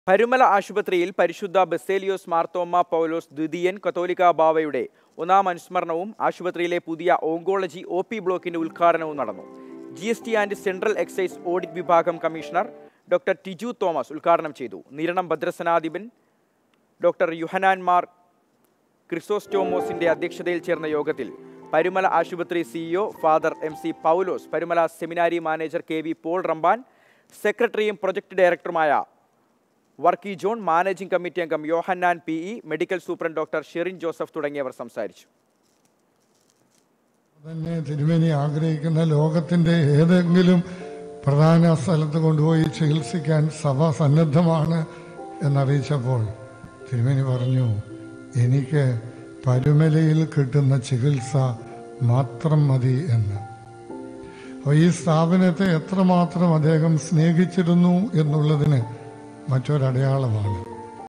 പൗലോസ് ദ്വിതീയൻ കാതോലിക്കാ ബാവായുടെ ഒന്നാം അനുസ്മരണം പരുമല ആശുപത്രിയിൽ നടന്നു | Mathrubhumi News
പരുമല ആശുപത്രിയിൽ പരിശുദ്ധ ബസേലിയോസ് മാർത്തോമ്മാ പൗലോസ് ദ്വിതീയൻ കാതോലിക്കാ ബാവായുടെ ഒന്നാം  അനുസ്മരണവും, ആശുപത്രിയിലെ പുതിയ ഓങ്കോളജി ഓ.പി ബ്ലോക്കിന്റെ ഉദ്ഘാടനവും നടന്നു.

Happy viewing!